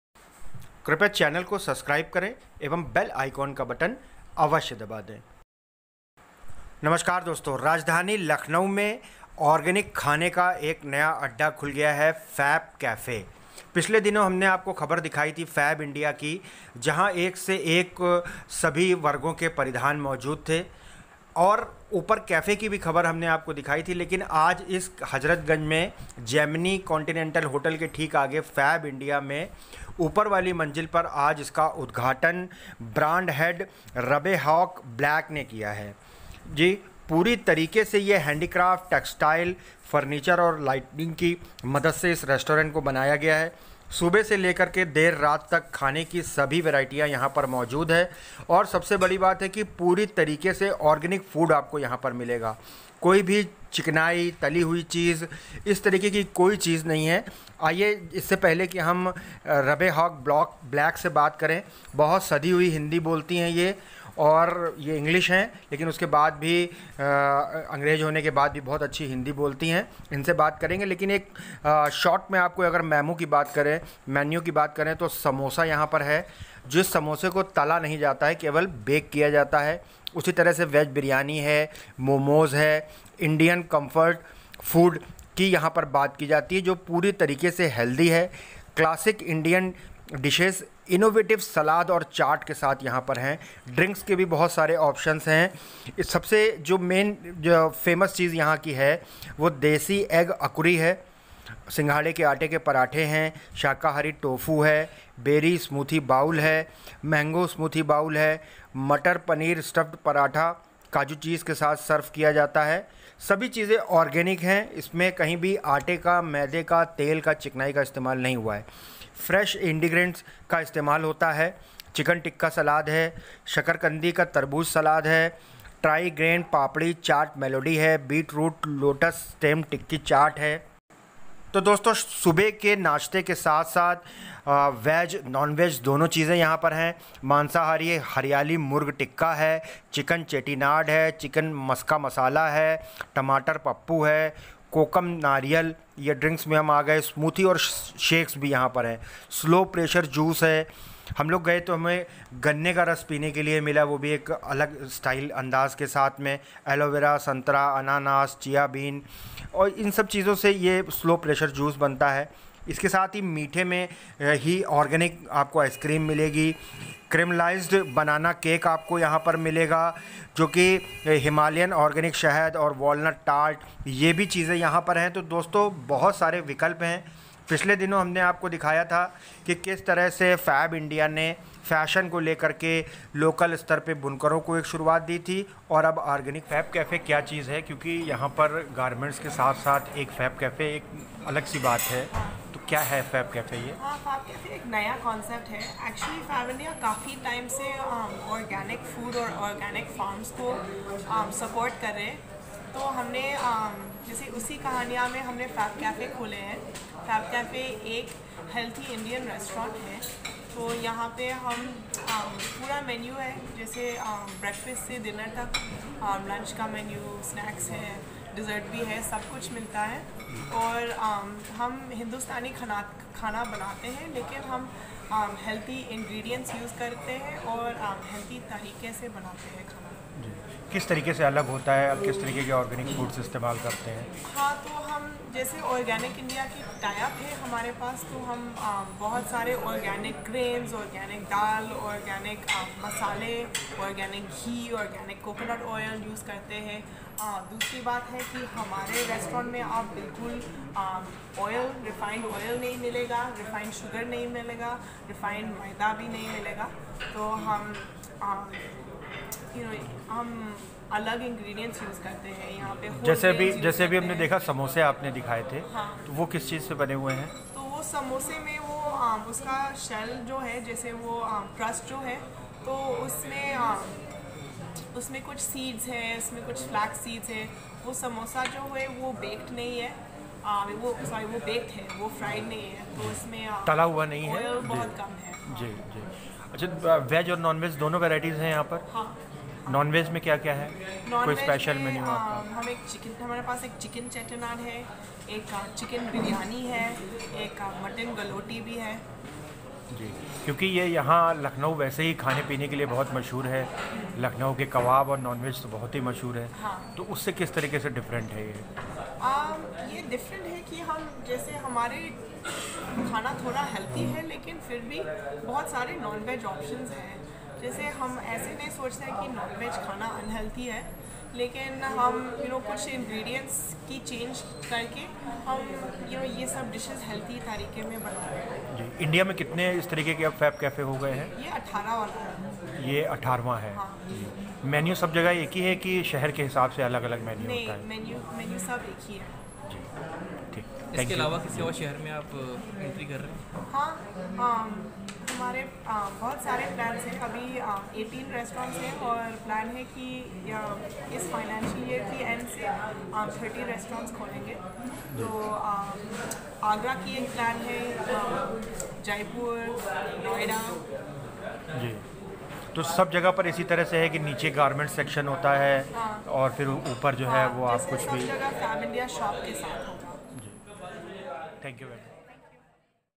फैबकैफे; बिना तला चिकनाई से मुक्त खाने का नया अड्डा
national watch news lucknow, fabindia's fabcafe now opened and serving organic food in lko, from breakfast, lunch and dinner, vegetarian and non vegetarian many menu in restaurant, healthy Indian food without oil. drinks, coffee, soups, juice.
नवाबों के शहर में फैबकैफ़े बाय फैबइण्डिया की शुरुआत
लखनऊ। पुरे दिन स्वास्थ्यवर्धक भारतीय भोजन परोसने के कांसेप्ट लेकर फैबइंडिया द्वारा शुरू किया गया फैबकैफे नए-नए मार्केट में विस्तार कर रहा है। फूडीज की जन्नत माने जाने वाले हजरतगंज लखनऊ में फैबइंडिया एक्सपेरिएण्टियल सेंटर के फर्स्ट फ्लोर पर अब फैबकैफे खुला है।

वॉल टू वॉल 727 स्क्वेयर फीट जगह में बना फैबकैफे बहुत खुला और रोशनीदार है। बॉटनिकल वॉटरकलर इलस्ट्रेशंस इसके इंटीरियर को और खूबसूरत बनाता है। यह कैफे मॉर्डन होने के साथ ही फैबइंडिया के हेंडीक्राफ्ट टेक्सटाइल, फर्नीचर और लाइटिंग की मदद से भारतीय स्वरुप दिया गया है। फैबकैफे के मेनू में पुरे देश से क्लासिक इंडियन डिशेस, इनोवेटिव सलाद और चाट शामिल किया गया है। यदि आप ग्लूटेन फ्री, वेगन, कीटो या सिर्फ ग्लूटोन है तो भी आपके लिए फैबकैफे में ऑप्शंस है। रेस्टोरेंट हर दिन 8.30 बजे से रात 10.30 तक खुला रहता है यानि आप यहाँ अपने दोस्तों के साथ लंच पर या परिवार को लेकर डिनर करने आ सकते हैं, या फिर जब कभी मन करे एक कप कॉफी का लुत्फ लेने यहाँ आ सकते हैं। फैबकैफे में दिन के हर वक्त के लिए कुछ न कुछ है।

हम खाना पकाने के लिए इस्तेमाल करने वाले मसालों पर विशेष ध्यान देते हैं। हम खाना बनाने के लिए रिफाइन तेल, आटा और शक्कर का इस्तेमाल नहीं करते हैं। हम आर्गेनिक घी, पारम्परिक साबुत अनाज और मीठे के अन्य प्राकृतिक विकल्पों का उपयोग करते हुए खाना बनाते हैं। ये सभी चीजे हमें अपनी पार्टनर कंपनी आर्गेनिक इंडिया से मिलते हैं। हमारे द्वारा उपयोग किये जाने वाले ज्यादातर खाद्य पदार्थ आर्गेनिक है और आर्गेनिक इंडिया से ही लिए गए है। फैबकैफे के बारे में मुझे जो बात सबसे ज्यादा पसंद है वह है इसका ऑल डे कांसेप्ट, यहाँ दिन भर आपको कुछ न कुछ अच्छा मिल जाएगा, चाहे वह वेगन कैपेचीनो हो या फिर स्ट्रेट फूड से इंस्पायर्ड सलाद। हमेशा फैबकैफे में कुछ नया और हेल्दी ऑप्शन मिलता है।

फैबकैफे में ब्रेकफास्ट मेनू है, जो दिन भर उपलब्ध रहता है। इस मेनू में देसी एग अकुरी (इसमें  नरम अंडे को प्याज, टमाटर और धनिया के साथ तल कर सिंघाड़े के आटे के पराठे के साथ परोसे जाते हैं। यहाँ एक शाकाहारी टोफू भी उपलब्ध है), बेरी स्मूथी बाउल (इसमें हेल्दी स्मूदी बाउल में स्ट्रॉबेरी, रास्पबेरी, ब्लूबेरी और वेनिला के ऊपर मूसली और ताजा फलों दिए जाते हैं), मैंगो स्मूदी बाउल ( इसमें समर सीजन में भारत के प्रसिद्ध आमों को मूसली और ताजे फल के साथ सर्व किया जाता है, इसे मटर-पनीर स्टफ्ड पराठा और काजू-चीज के साथ सर्व किया जाता है। यह पराठा सिंघाड़े के आटे से बना होता है।)

सलाद सेक्शन इंडियन स्ट्रीट फूड से इंस्पायर्ड है। इसमें फ्रेश इंग्रीडिएन्स का इस्तेमाल कर लाइट मील या क्विक ऐपेटाइजर तैयार किए जाते हैं। इन विकल्पों में चिकन टिक्का सलाद (इसमें मिक्स ग्रीन्स, शिमला मिर्च, अचार प्याज को काजू पुदीने के साथ परोसा जाता है), शकरकंडी तरबूज सलाद ( इसमें भुना हुआ शकरकंद, तरबूज, मिश्रित साग, भुने हुए कद्दू के बीज, एक जिनी सिट्रस ड्रेसिंग के साथ परोसा जाता है)।

गेस्ट आमतौर पर ऐपेटाइजर के साथ अपने भोजन की शुरुआत करना पसंद करते हैं। वे ट्राई-ग्रेन पापड़ी चाट मेलडी (इसमें कुट्टू, ज्वार, बाजरा से बनी पापड़ी को दही, इमली की चटनी के साथ सर्व किए जाता है), बीटरूट लोटस स्टेम टिक्की चाट (इसमें सुपारी और कमल के साथ बनाई जाने वाली सुपर फूड टिक्की को )

दही, चटनी और लोटस चिप्स के साथ परोसा जाता है, इसमें शाकाहारी विकल्प भी उपलब्ध है),  बेक्ड दाल समोसा को चटनी ट्रायो के साथ सर्व किया जाता है  (या एक पारम्परिक व्यंजन का हेल्दी रूप है)। मांसाहारी हरियाली मुर्ग टिक्का (भारतीय मसालों से चिकन, पुदीना और धनिया के साथ बनाया चिकन टिक्का) चुन सकते हैं या एक मिनी कुट्टू ब्रेड पर मटन गलौटी चुन सकते हैं, जिसमे मुंह में रखते ही घुल जाने वाले नरम और स्वादिष्ट कबाब परोसे जाते हैं।

मैन कोर्स में नॉन वेजीटेरियंस चिकन चेट्टीनाड (नारियल, नट्स और15 भुने हुए मसालों के साथ तैयार सुगंधित करी परोसी जाती है), चिकन मस्का मसाला (टमाटर मखनी ग्रेवी में बना रसीला मसालेदार चिकन) और टमाटरपप्पू (इसमेंसाधारण  दाल को नारियल तेल, टमाटर के साथ साउथ इंडियन हर्ब्स के साथ पकाया जाता है), कथल क्विनोआ बिरयानी (इसमें लाइट बिरयानी को कटहल, रायता और सालन के साथ परोसा जाता है) जैसे विकल्पों को चुन सकते हैं। डिनर स्पेशल में पारसी चिकन धांसक खास है जिसमे मौसमी सब्जियों के साथ दाल में चिकन धीमी गति से पकाया जाता है। इसके साथ ही लखनवी काजू मटन कोरमा भी बेहद खास डिश है, जिसमे बोनलेस मटन को केसर के साथ कोरमा ग्रेवी में पकाया जाता है।

फैबकैफे का ड्रिंक मेनू पारंपरिक शीतल पेय जैसे आम पन्ना (मसालों और कच्चे आम से बना) के साथ शुरू होता है। इस मेनू में कोकम नारियल (नारियल के दूध, ताड़ के गुड़ और कोकम से बना) भी शामिल है। फैबकैफे के पास स्मूथी और शेक्स भी है, जिन्हे ताजे फलों और प्राकृतिक सामग्री से बनाया जाता है। इसके साथ ही कोम्बुचा से चुनने के लिए भी विकल्प है। इसके साथ ही फैबकैफे ने स्लो प्रेशर जूस भी इंट्रोड्यूस किया है जिसमे नारियल पानी, एलोवेरा, संतरा, अनानास और चिया बीज से बना ग्लो हाइड्रेशन शामिल है। फैबकैफे के कॉफी मेनू में देश की कुछ बेहतरीन कॉफी शामिल हैं।